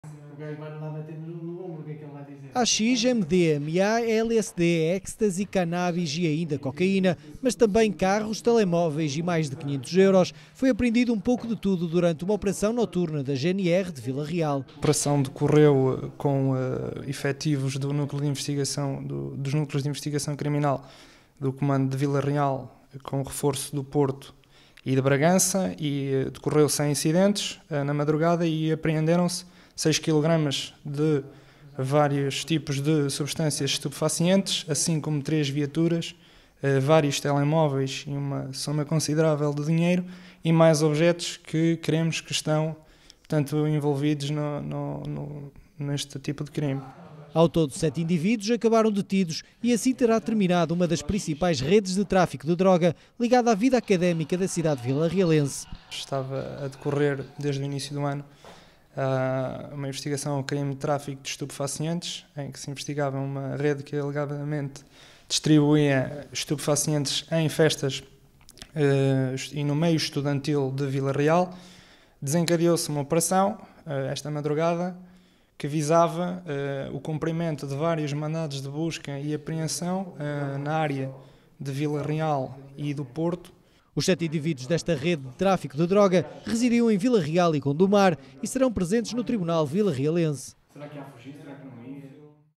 Vila Real, GNR deteve 7 indivíduos por tráfico de droga
uma rede que actuava durante as festas nocturnas da vida académica da cidade de vila real. é o que a GNR pensa ter desmantelado durante a madrugada de hoje com uma operação que envolveu mais de 30 agentes e buscas em várias residências. sete homens foram detidos e serão presentes no tribunal de vila real durante o dia de amnahã

Local: vila real

oráculo:
detidos pertenciam a rede de tráfico ligada à vida académica

Fala:

Perdo Reis _  Alferes GNR de Vila Real

Repórter de Imagem: Luís Ruas
Jornalista: Frederico Correia